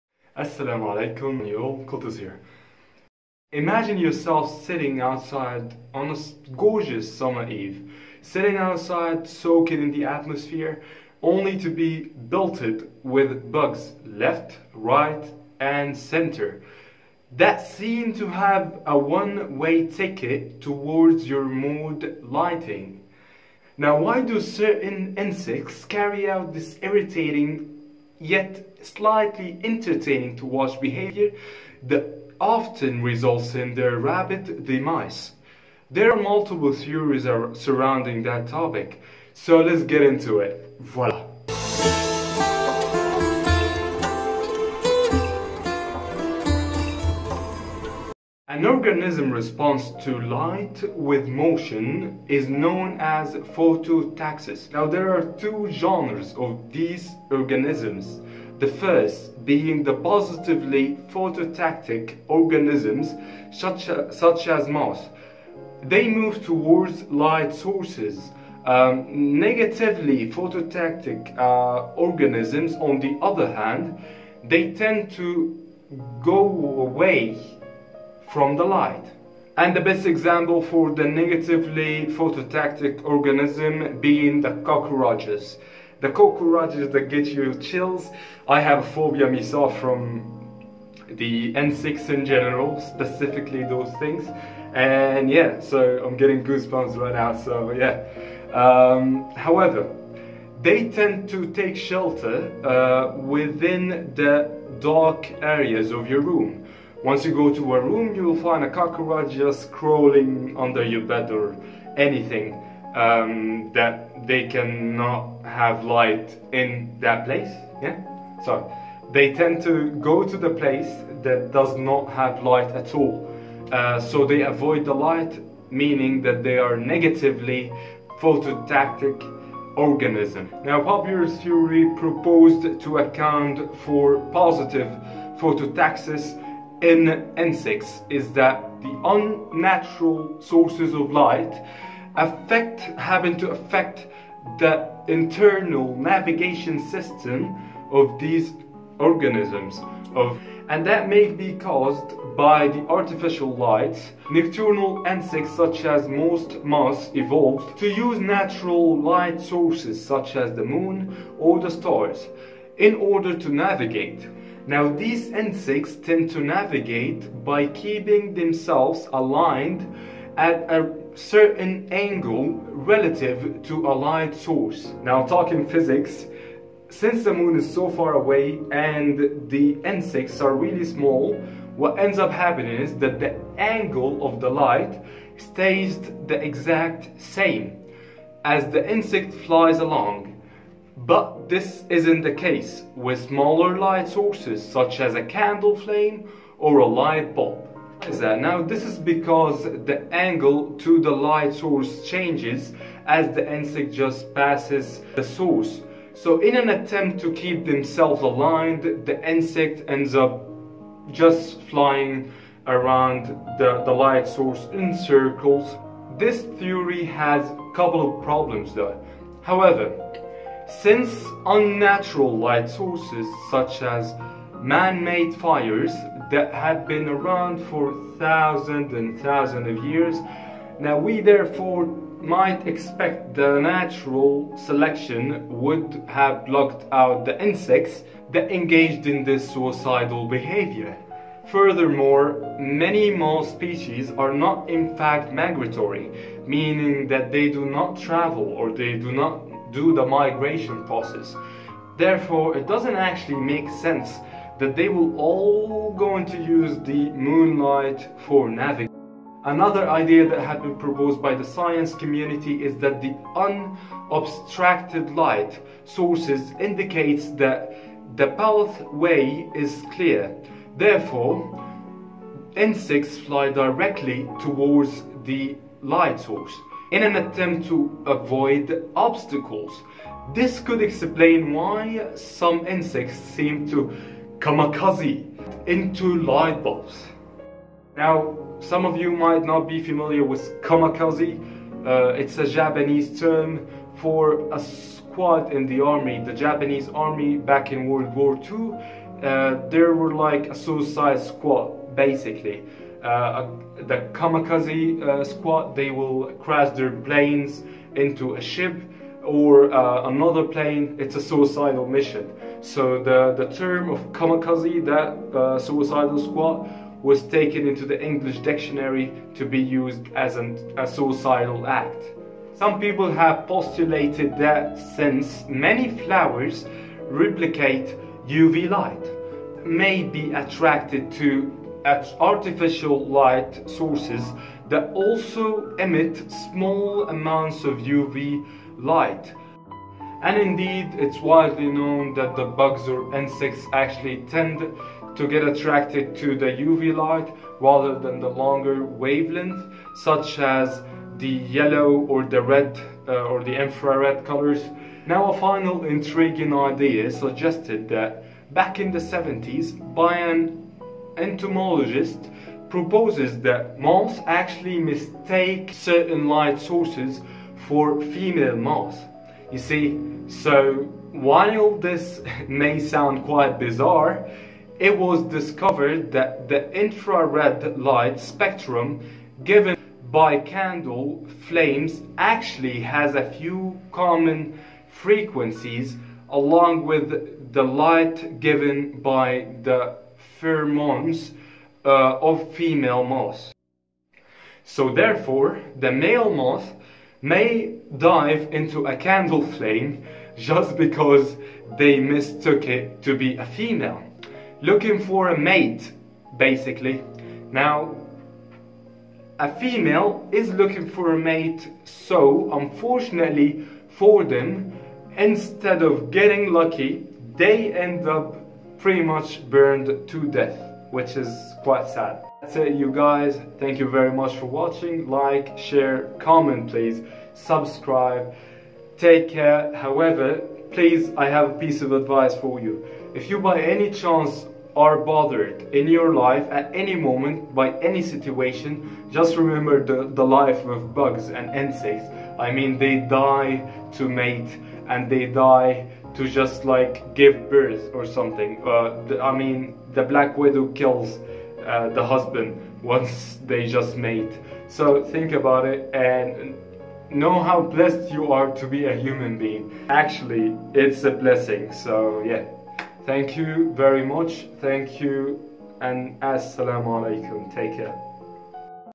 Why do insects fly into light?
Educational video about moths and why do they fly towards the light!